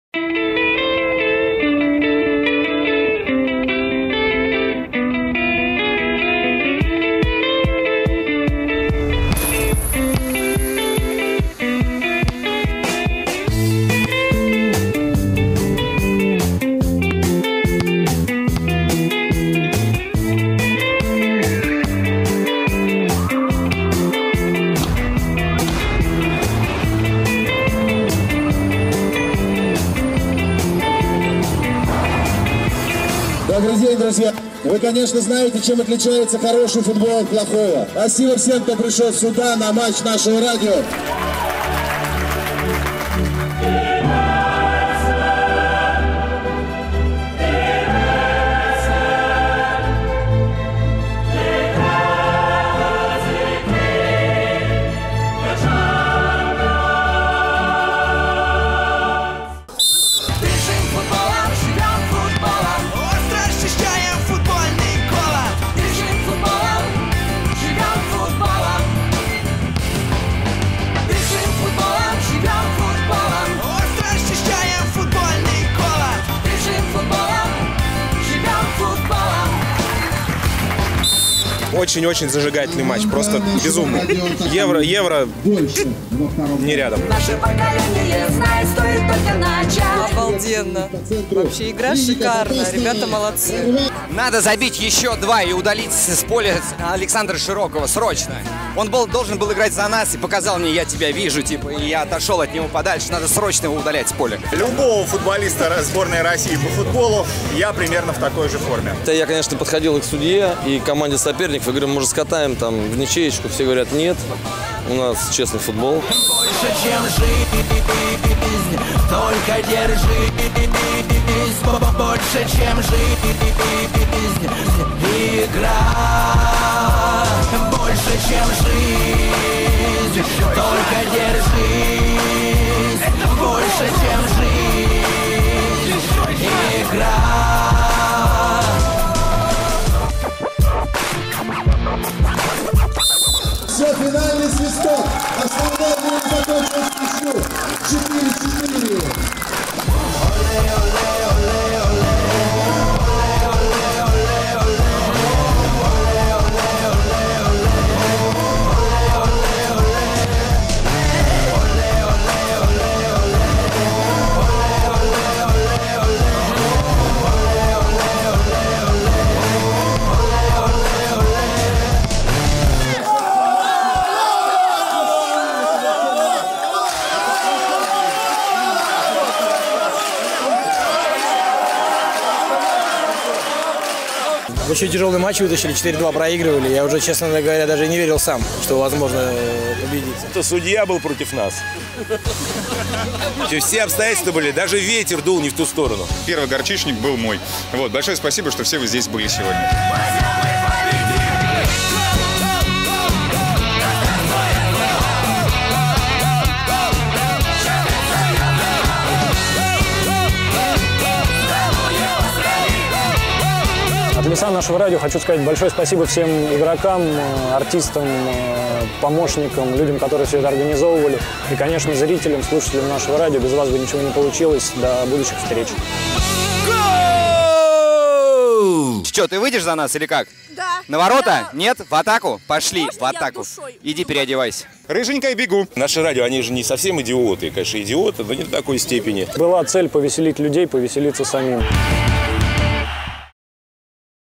Матч за Кубок НАШЕго Радио
12 сентября состоялся первый матч за "Кубок НАШЕГо Радио". Команда НАШЕго Радио не на жизнь,а  на смерть билась с командой НАШИх Артистов. В равной борьбе победило НАШЕ Радио. Спасибо всем, кто был. И представляем вам видеорепортаж о том, как это было.